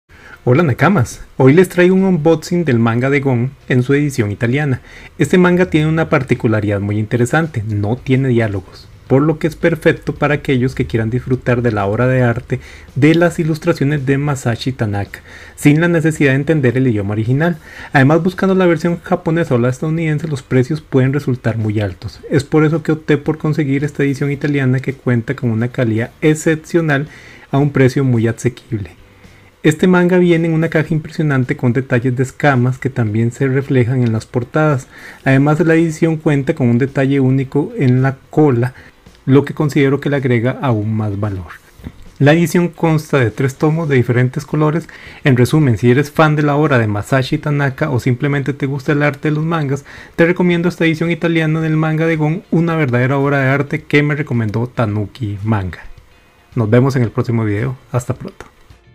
GON ¿Manga sin diálogos?/Unboxing.
¡Hola nakamas! Hoy les traigo un unboxing del manga de GON en su edición italiana. Este manga tiene una particularidad muy interesante: ¡no tiene diálogos! Por lo que es perfecto para aquellos que quieran disfrutar de la obra de arte de las ilustraciones de Masashi Tanaka sin la necesidad de entender el idioma original.

Además, buscando la versión japonesa o la estadounidense, los precios pueden resultar muy altos. Es por eso que opté por conseguir esta edición italiana, que cuenta con una calidad excepcional y a un precio más asequible.

Este manga viene en una caja impresionante con detalles de escamas que también se reflejan en las portadas. Además, la edición cuenta con un detalle único en la cola, lo que considero que le agrega aún más valor.

La edición consta de 3 tomos de diferentes colores, y en esta reseña, les mostraré un poco de cada uno de ellos. Cada tomo tiene hojas a color y las portadas tienen ese mismo efecto de escamas.

En resumen, si eres fan de la obra de Masashi Tanaka o simplemente te gusta el arte de los mangas, te recomiendo esta edición italiana del manga de GON. Una verdadera obra de arte que no te puedes perder.